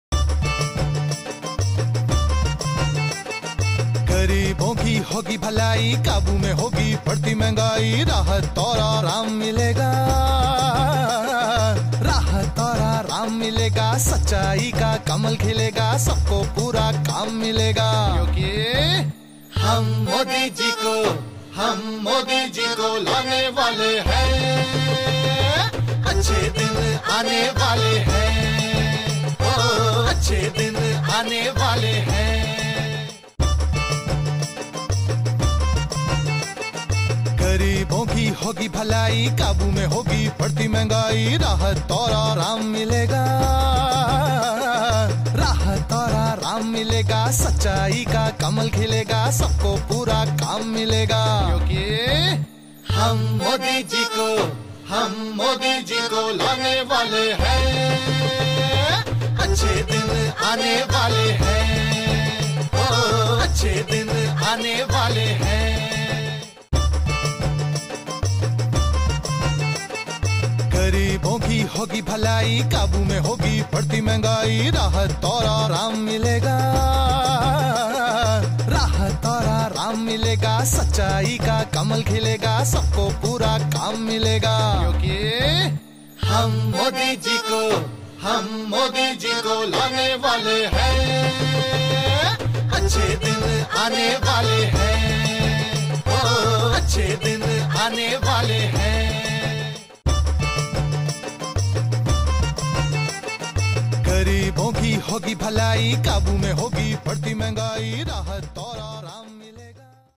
Moti singh (मोती सिंह) parivartan Railly in Saifabad Patti PRATAPGARH
कैबिनेट मंत्री माननीय राजेंद्र प्रताप सिंह परिवर्तन रैली सैफाबाद पट्टी प्रतापगढ़ में।।।।